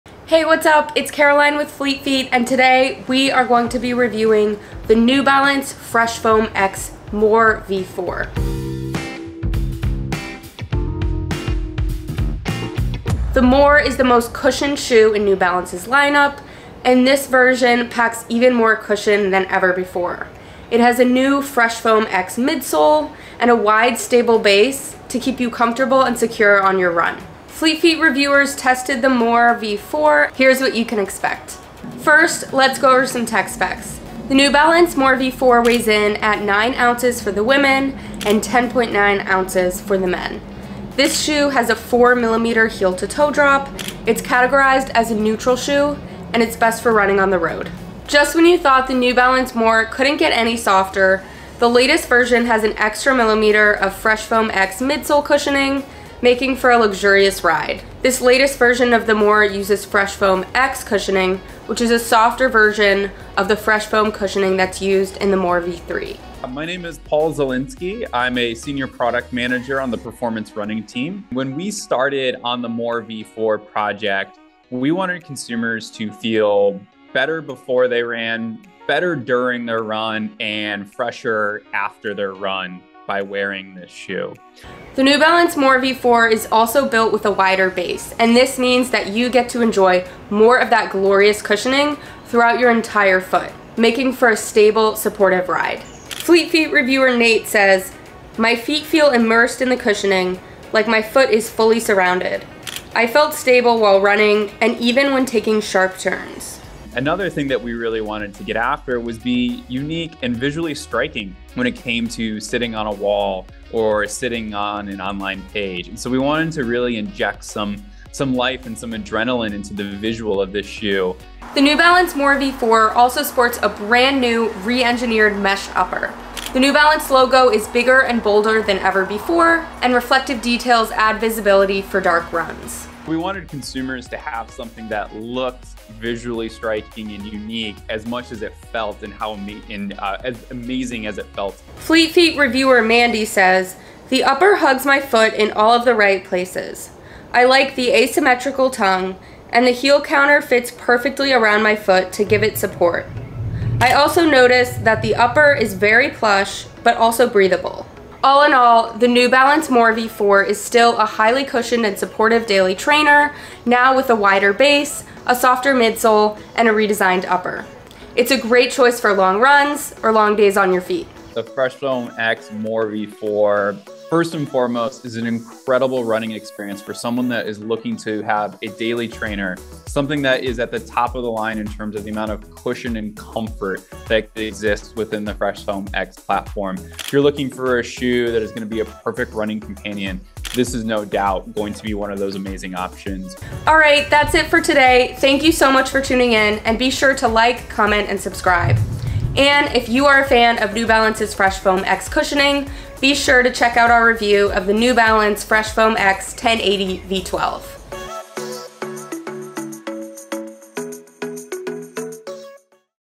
Shoe Review: New Balance Fresh Foam X More v4
The More is the most cushioned shoe in the New Balance lineup, and the latest version packs even more plushness than ever.